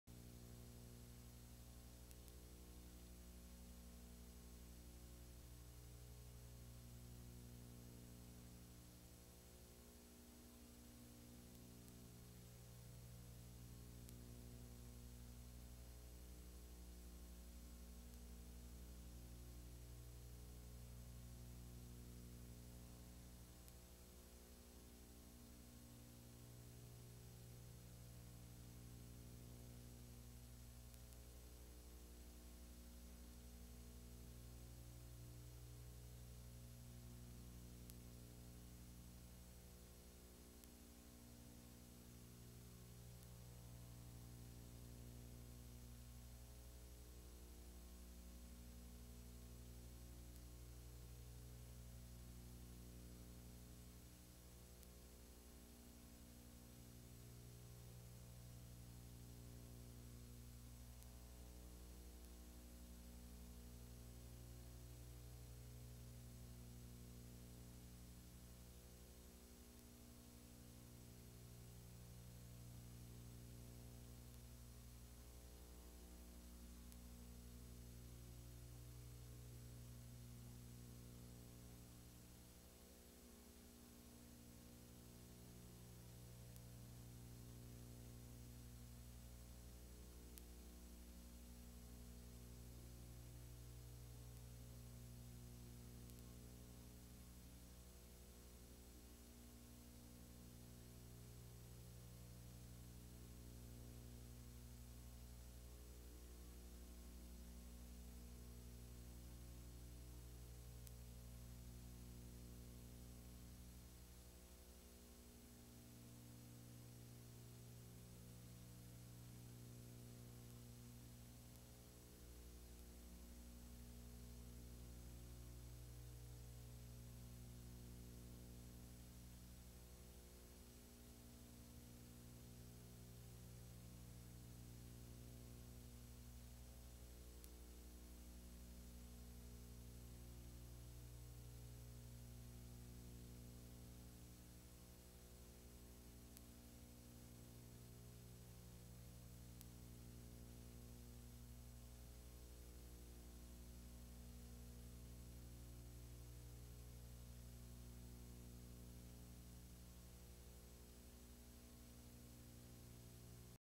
1987 Adams Cup Race 1 (No Sound)
First of four Adams Cup races on the 1987 season tape.  Note: none of the cup races on this tape had sound, but the Eastern Sprints and Henley footage did have sound.  We aren't sure which event (1F, JV, V, etc) each of these is, so we'd appreciate confirmation from someone with better information.